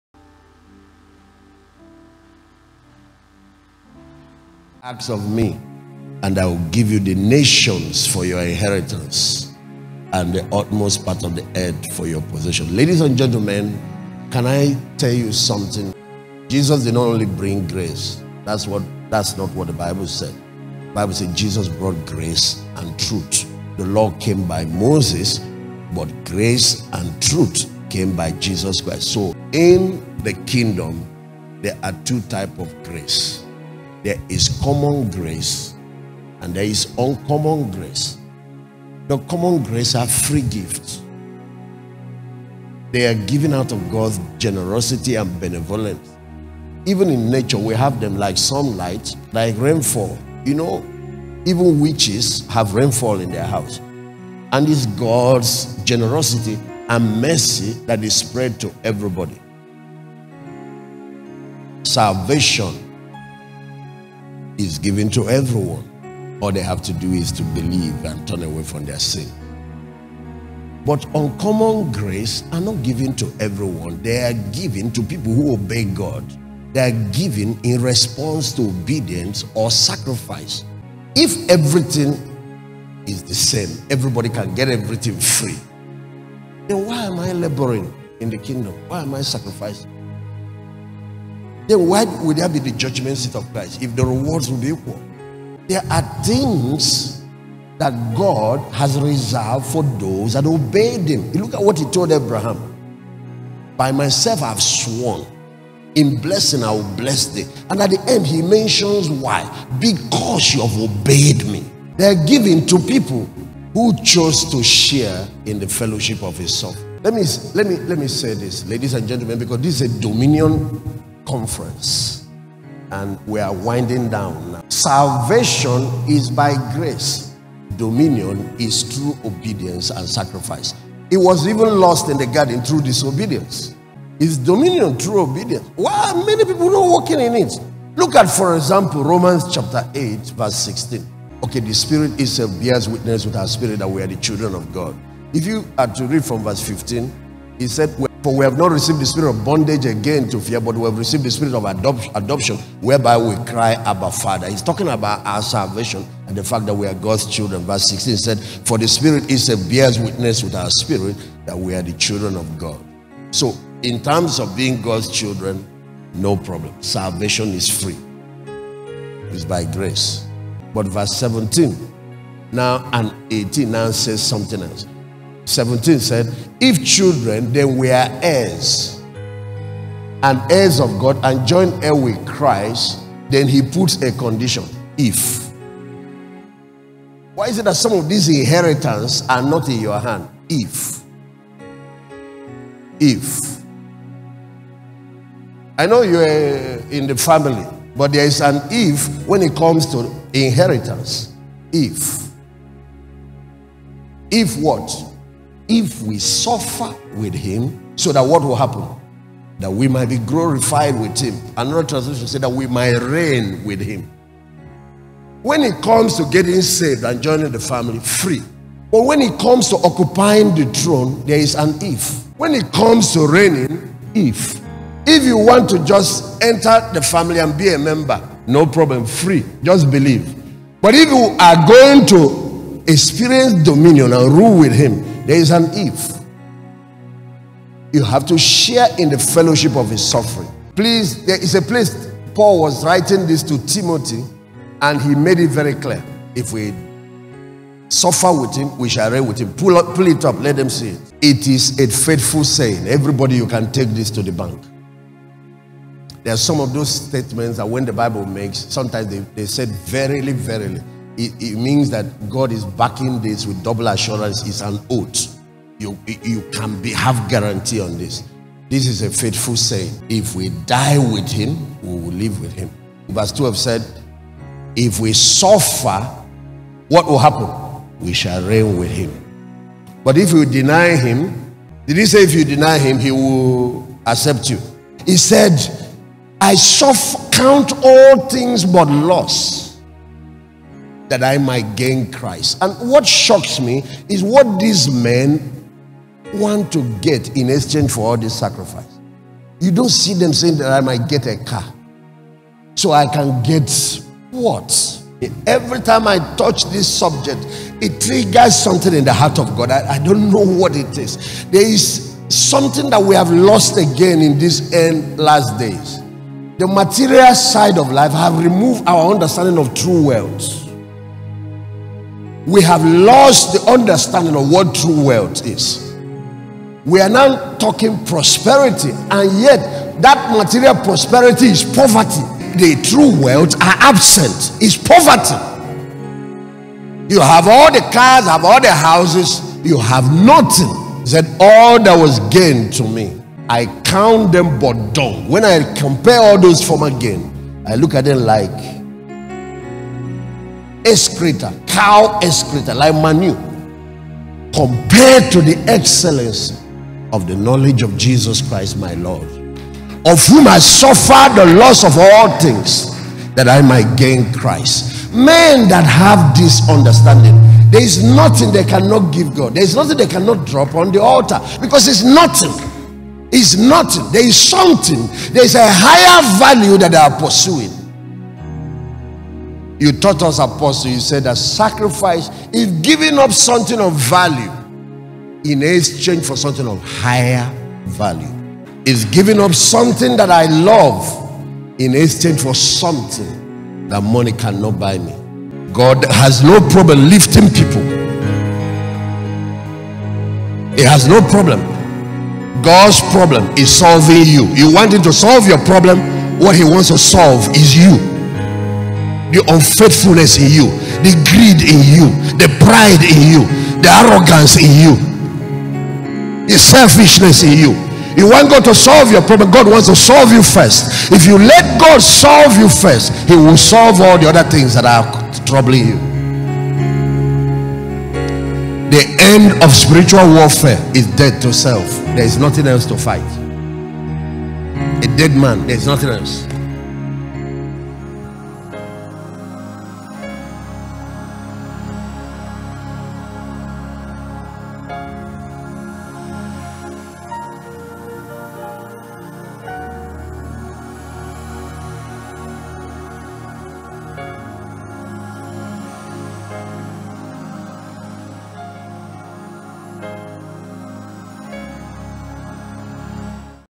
THE IS THE SECRET TO TRUE RICHES - DR. DAVID OGBUELI
What is the bridge between us and true riches? Have we been sidetracked by other less important things?

Dominion City was founded by Pastor David Ogbueli.
The mandate is to raise leaders that transform society.